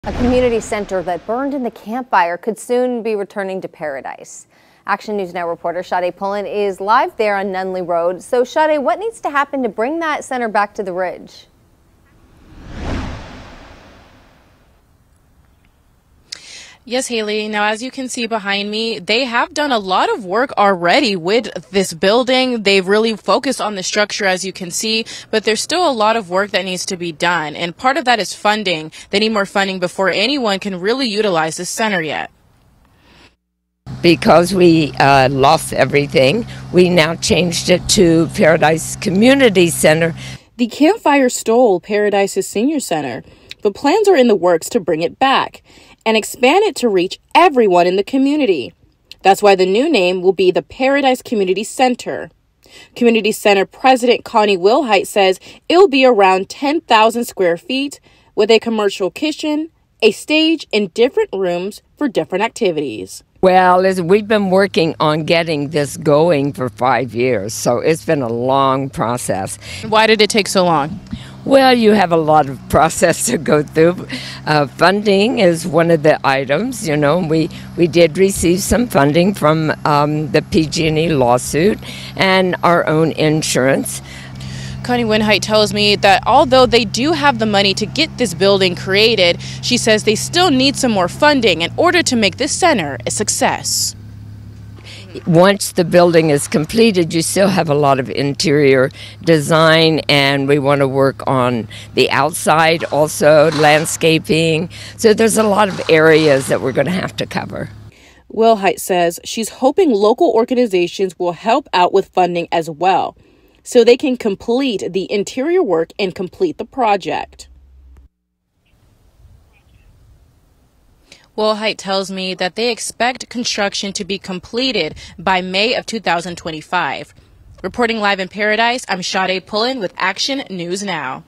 A community center that burned in the Camp Fire could soon be returning to Paradise
Action News Now reporter Sade Pullen tells us more about the progress of the center.